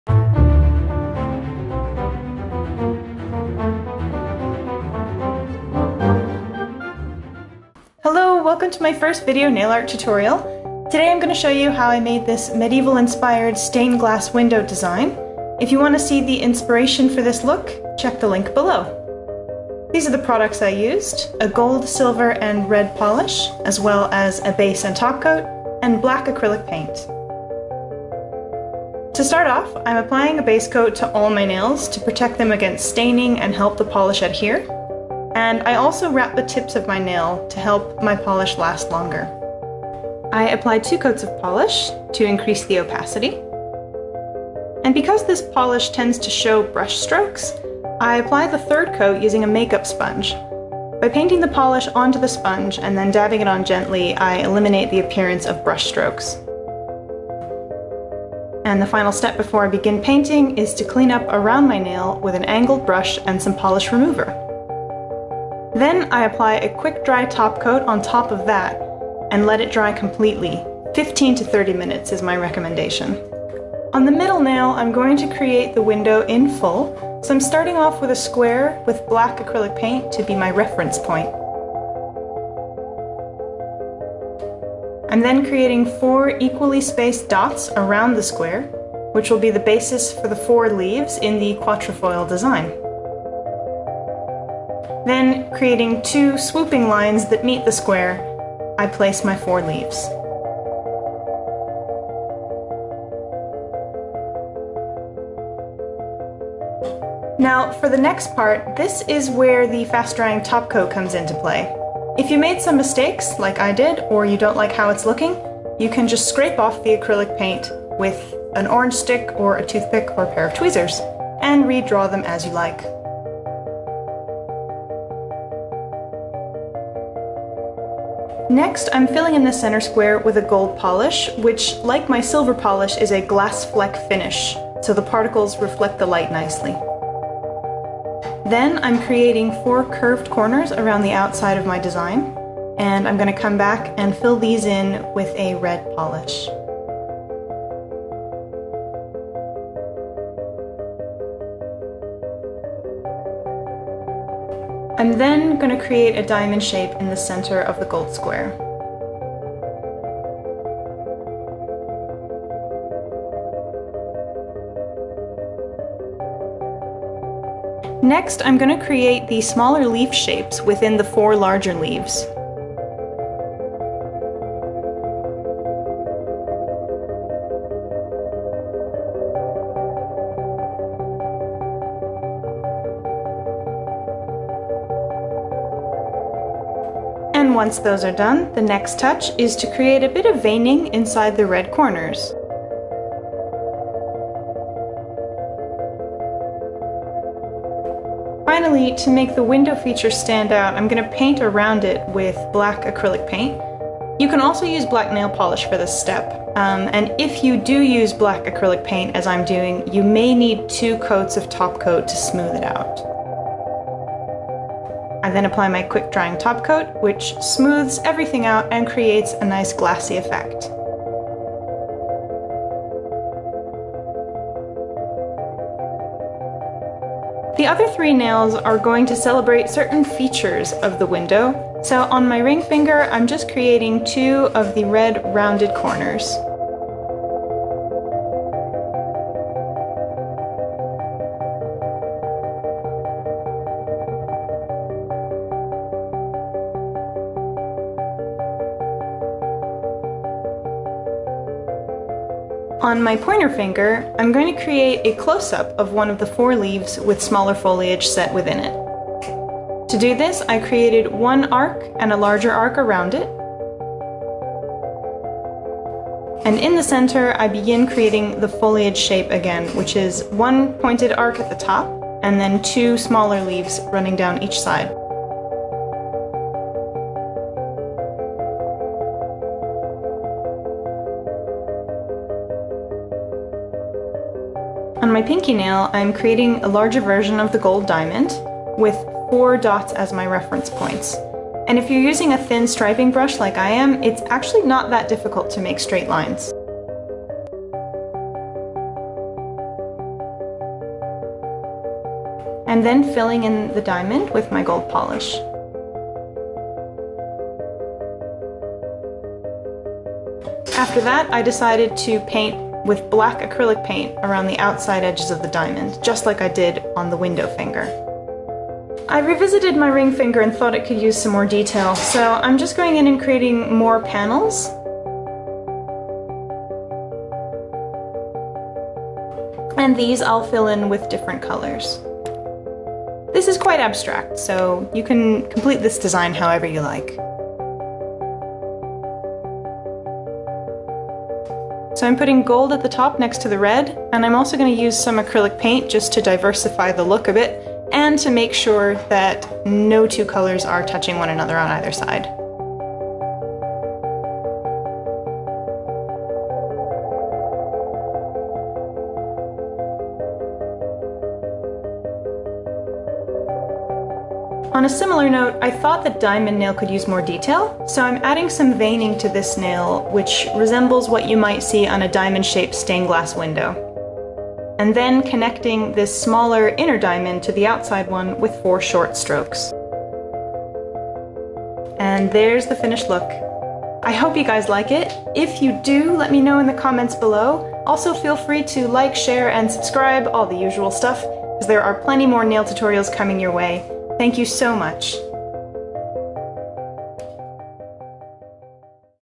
Medieval Stained Glass nail art tutorial
For my first tutorial (ever!) I've created a look inspired by a 13th century stained glass window feature. The full detail is on my middle nail and I added leaf and diamond motifs from it to my other nails. I hope you enjoy it. If you decide to recreate my design, tag @marginailia on Instagram!

Also, thumbs up if you liked Ushi the dancing degu ^.^

If you want to geek out over actual authentic medieval stuff (I trained to be a medievalist, so I will like you LOTS if you do), check out the original French quatrefoil window detail that inspired me Learning is fun, kids!

Products used:
Zoya Ziv (gold)
Zoya Trixie (silver)
Zoya Blaze (red)
Black acrylic paint
Seche Clear base coat
Cheaposaurus detail brush and striping brush form Amazon (that's not a brand. Don't waste minutes of your life looking up "Cheaposaurus" on Amazon... or do, and report back,
pls).

Zoya polishes used in the intro, from left to right: Elisa, Thandie, Ziv, Midori, Yara, Rocky, Ling, Charisma, Mimi, Sansa

I film using a Sony a6000 and edit with Adobe Premiere Pro CS6.